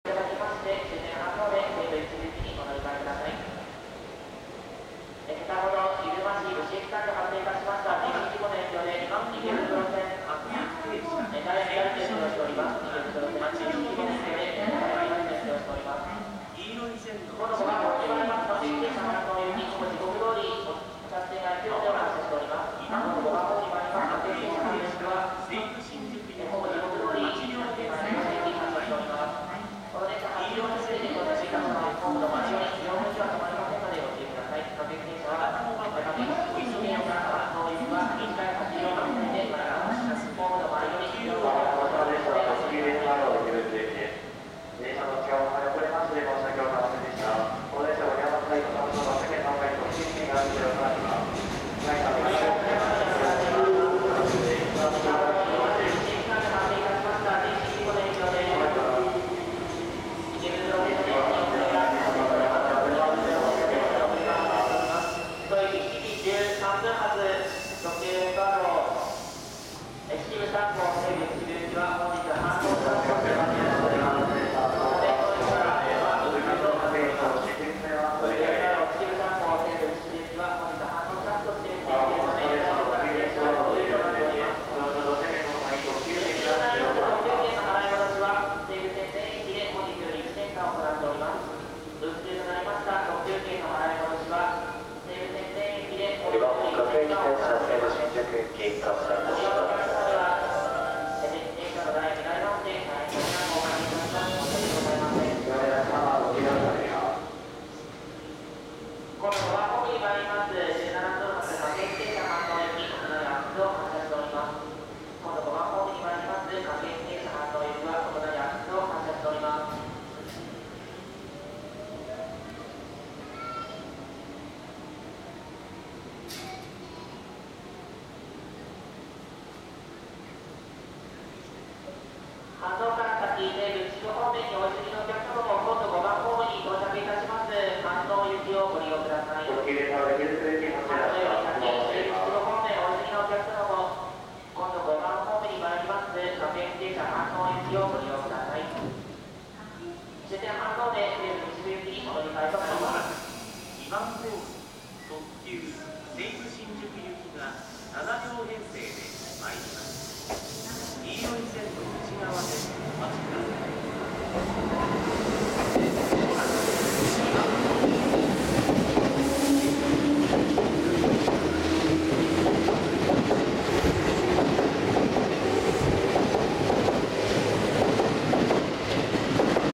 １００００系西武池袋線特急むさし？号池袋行き所沢駅入線～発車
２０１５．５．４撮影です。